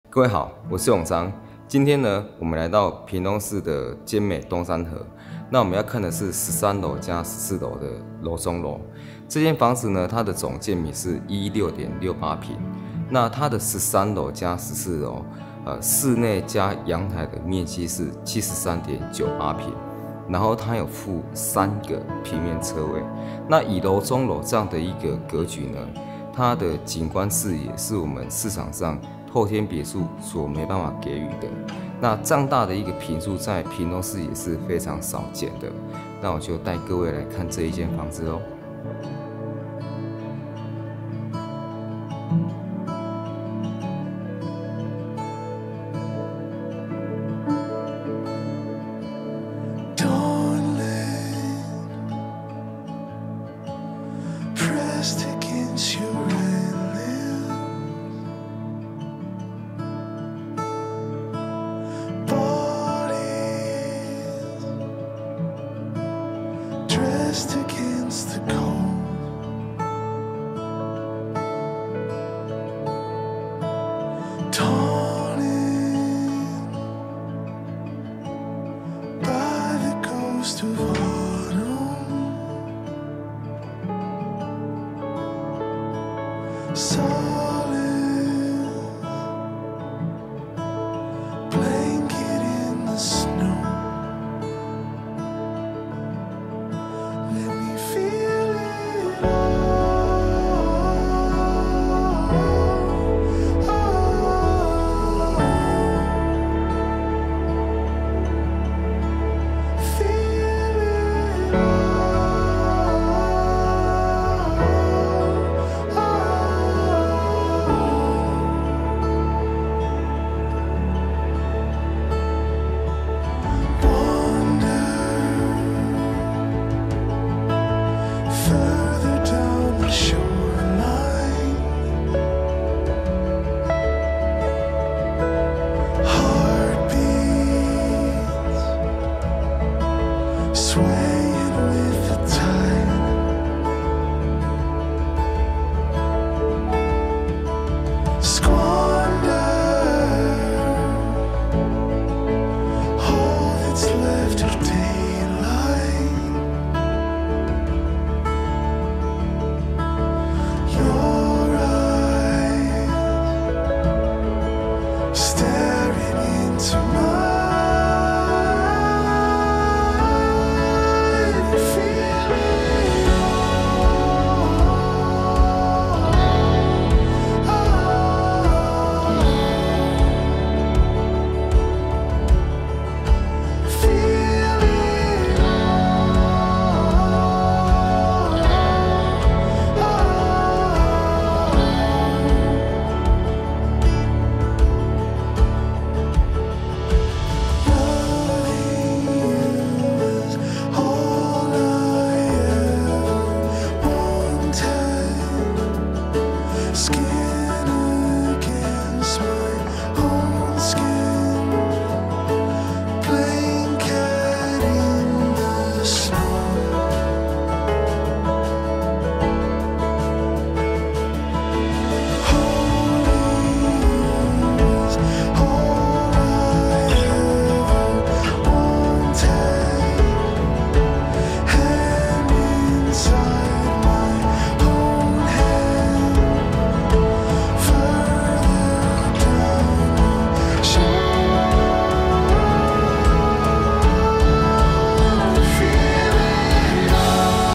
（已售出）寬，宅II。屏東市尖美東山河樓中樓｜售價1088萬，總建坪116.68坪，附三個平面車位
寬，宅II。屏東市尖美東山河樓中樓｜售價1088萬，總建坪116.68坪，附三個平面車位
家，幸福的開始｜屏東房地產 買屋｜賣屋 陳泳璋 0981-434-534。
營業員證號：（103）登字第283183號
永慶廣東店/琦健不動產股份有限公司
經紀業：屏府地價字第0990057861號